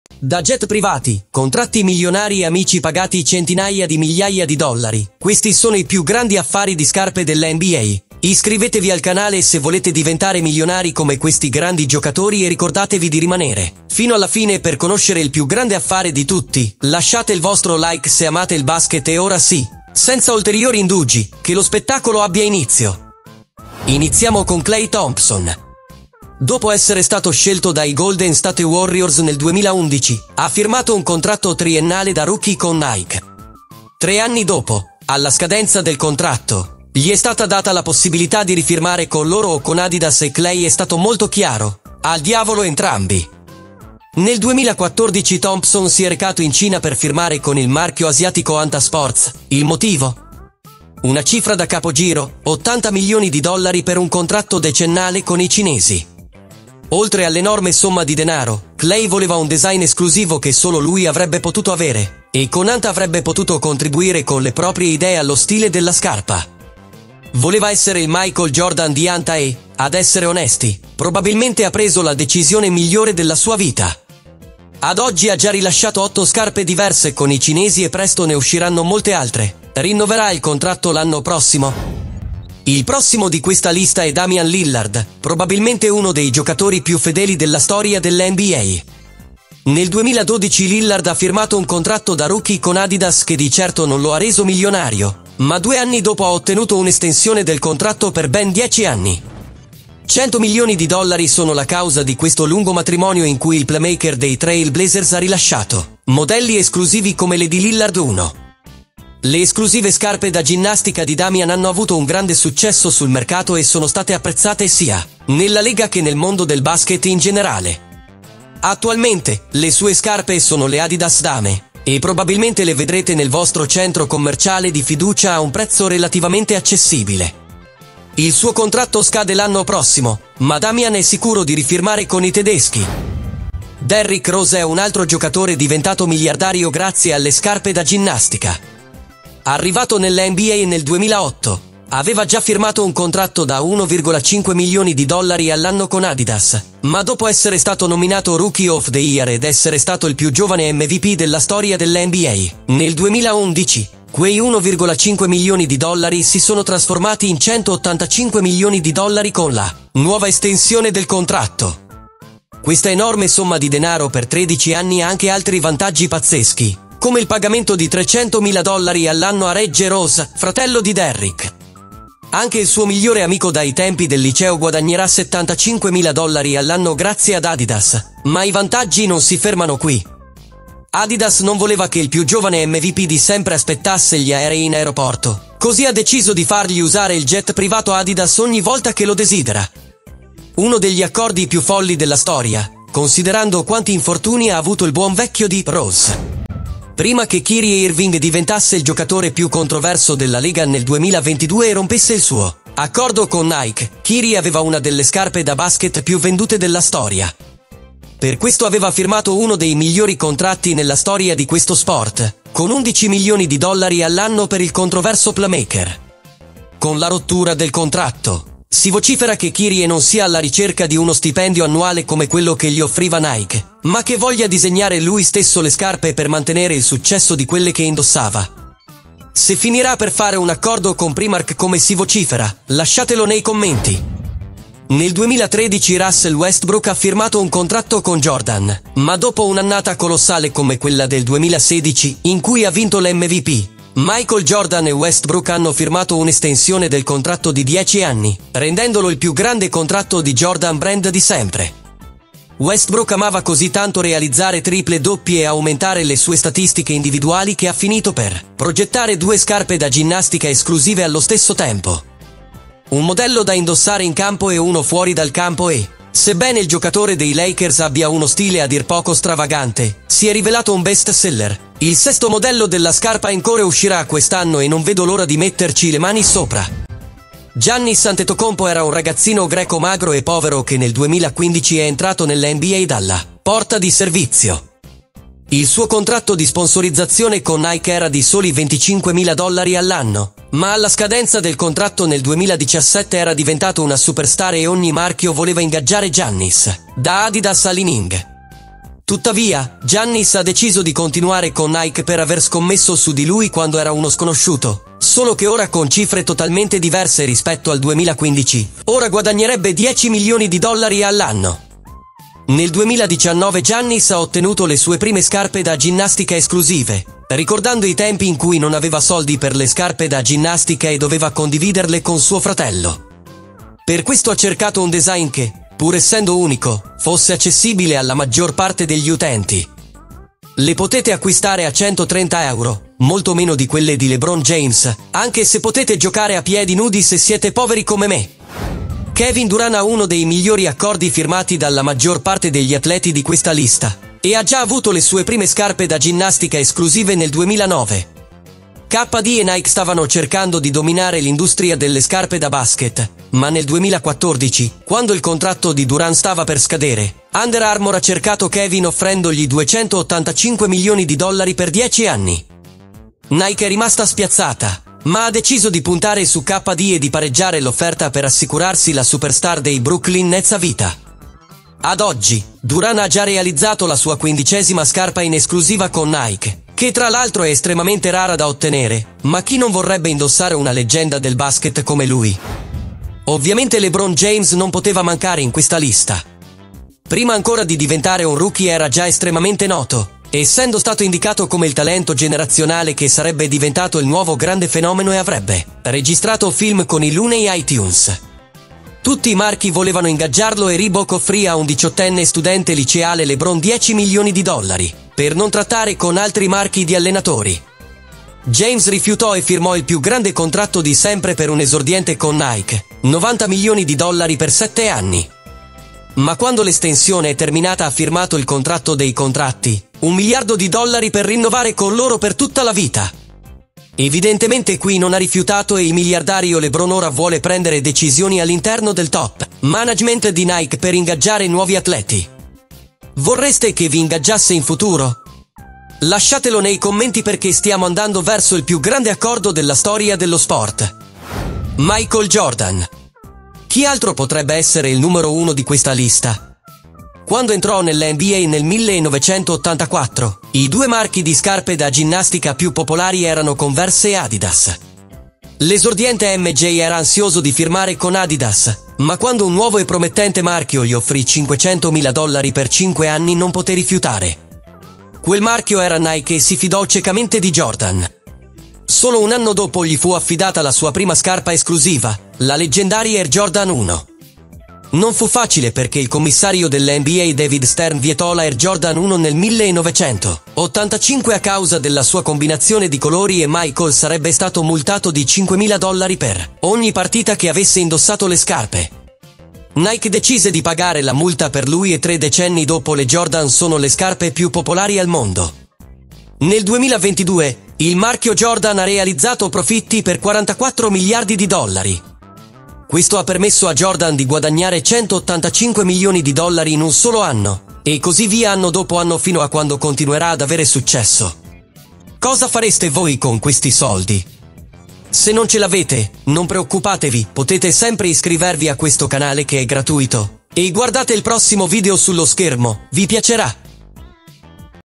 🤑 I PIÙ GRANDI Accordi Pubblicitari di Scarpe nella NBA
Nel video di Tiro da Tre di oggi, ci occuperemo di tutti i più grandi contratti di scarpe dell'NBA! Questi giocatori hanno contratti bloccati che valgono milioni di dollari con i principali marchi di scarpe come Nike, Adidas, Jordan, Li Ning e molti altri! Date un'occhiata agli altri video per vedere come tutti questi giocatori NBA spendono i loro milioni!